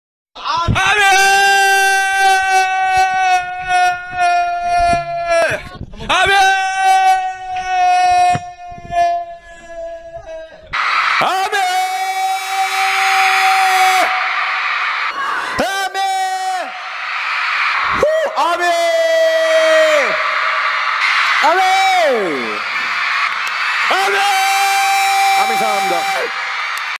ARMY!!!!!!!! | BTS Screams
"ARRRMMMYYYY!" LMAO I will never be tired because of them! 😂